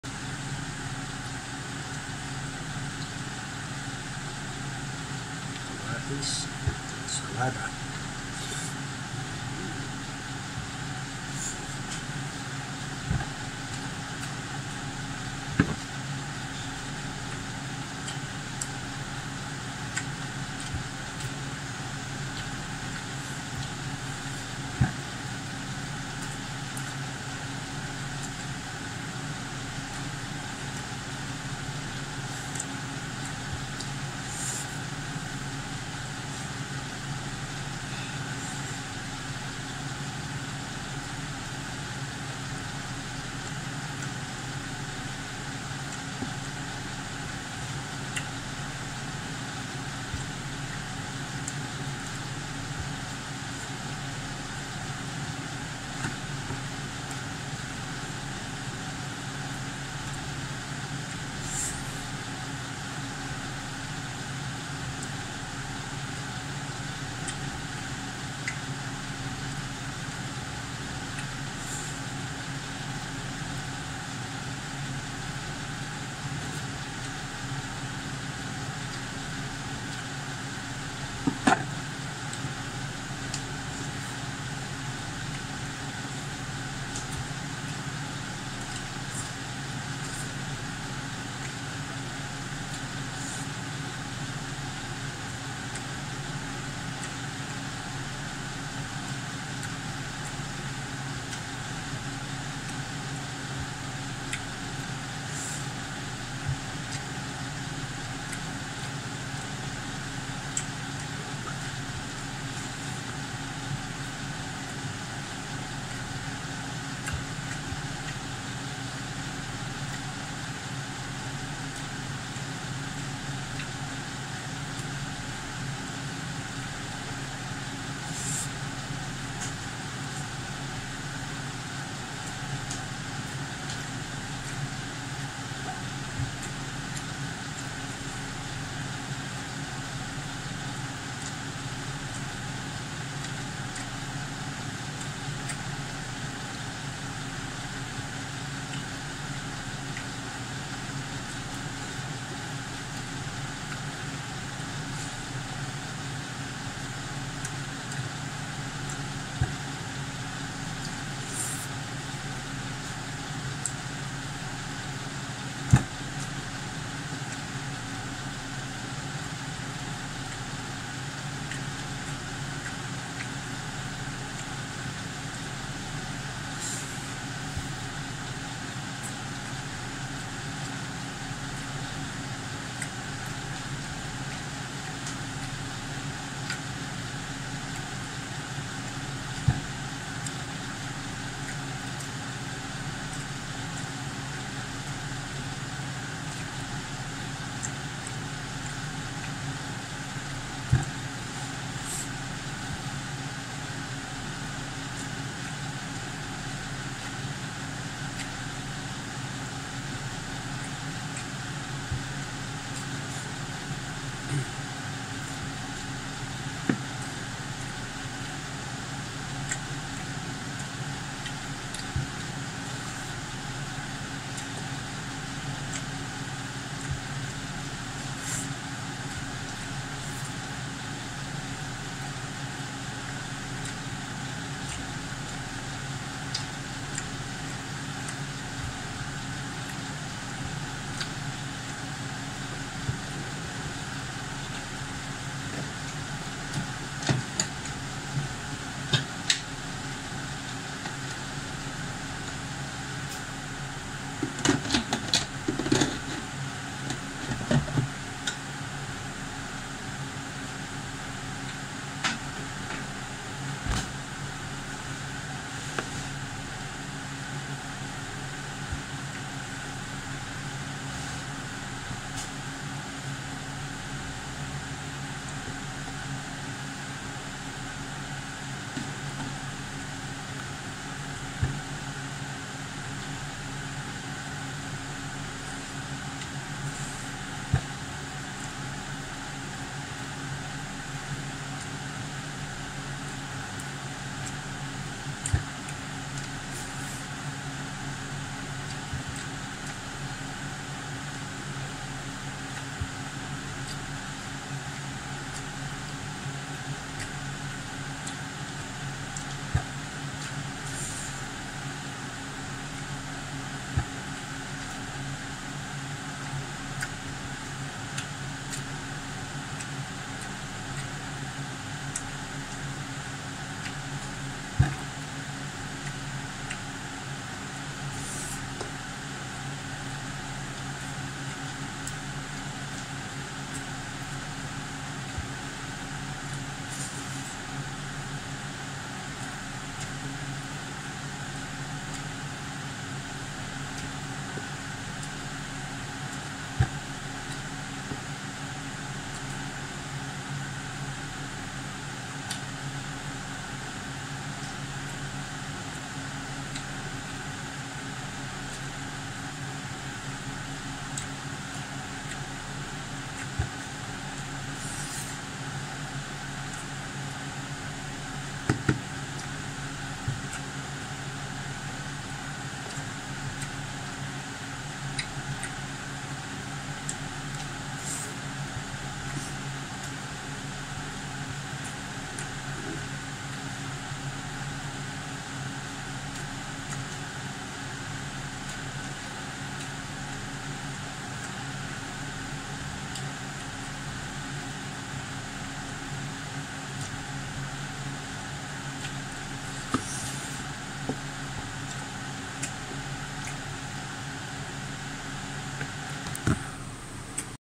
ASMR MUKBANG |KALDERETANG BAKA
Halina at kumain ng mainit na kanin na may kasamang ensaladang tomato, Kaderetang baka na mainit init bagong luto, at dahil gutom na gutom na ako dahil pagod sa work mag hapon kaya ayan at panoorin po paano kumain ang isang trabahador at simpelng tao kamay kamay kamay tayo ,  halos naka 3 cups na white rice po ako at masarap kumain kapag galing ng work  ang sarap ng kalderetang baka at malambot na kanin plus tomato salad ayos salamat po sa Dios.